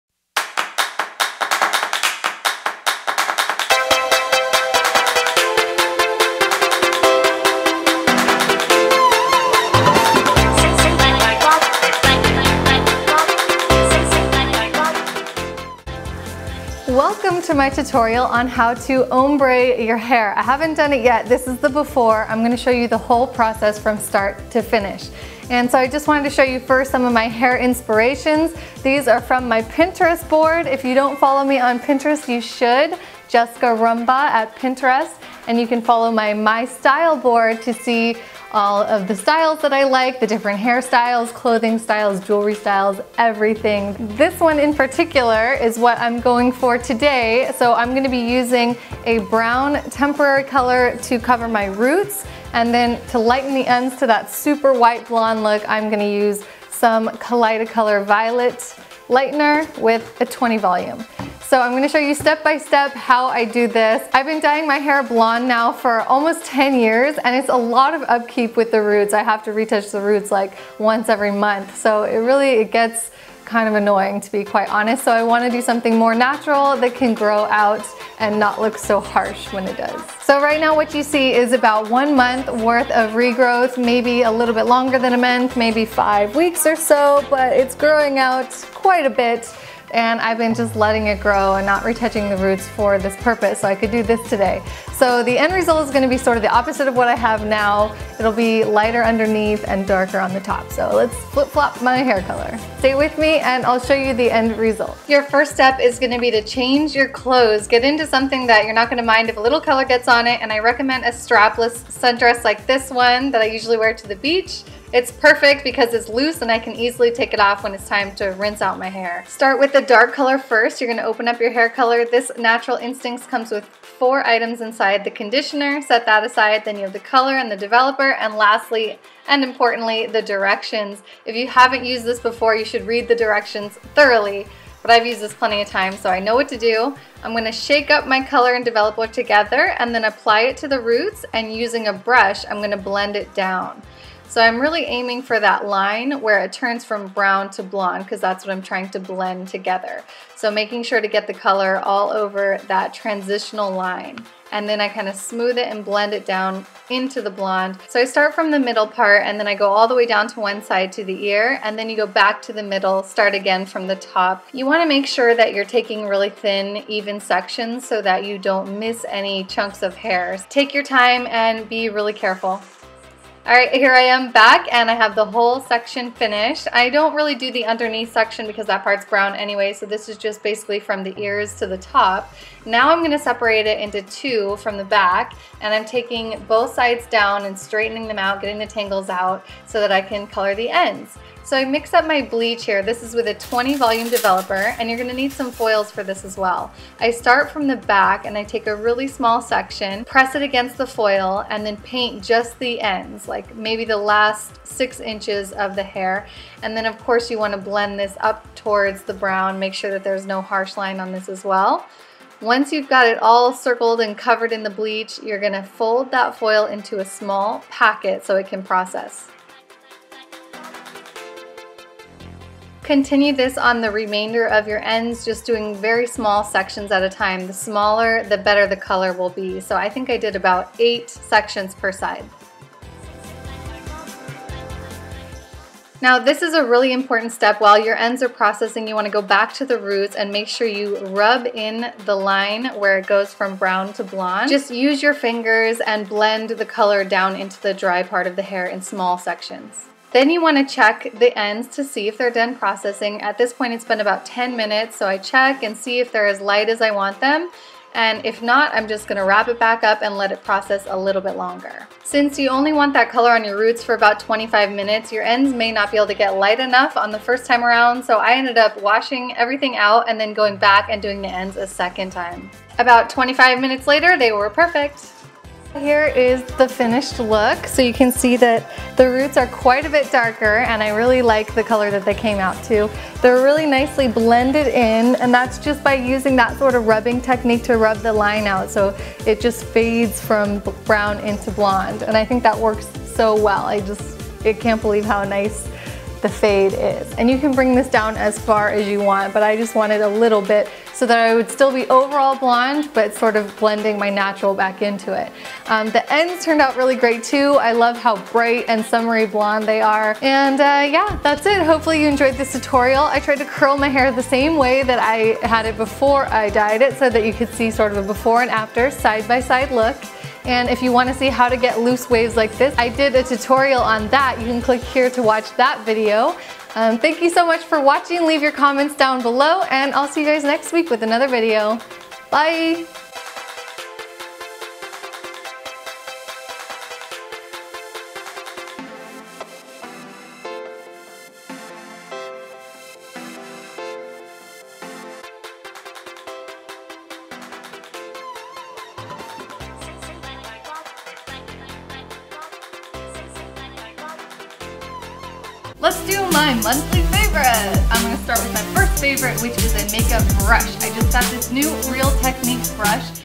DIY Blonde Ombré Hair Color
Here's how I give myself an Ombre-effect while still keeping primarily blonde, all done at home!
To create this look you will need:
-Dark Blonde or Brown semi-permanent hair color. I love Clairol's Natural Instincts.
Here's the one I used
-Powder Lightener, I love Kaleido Color in violet
-20 Volume Developer (this is the strength I recommend to keep your hair healthy and strong)
Here's the link to the one I like
-2 color brushes
-2 color bowls
-Foil

Follow the steps in the video to achieve this look!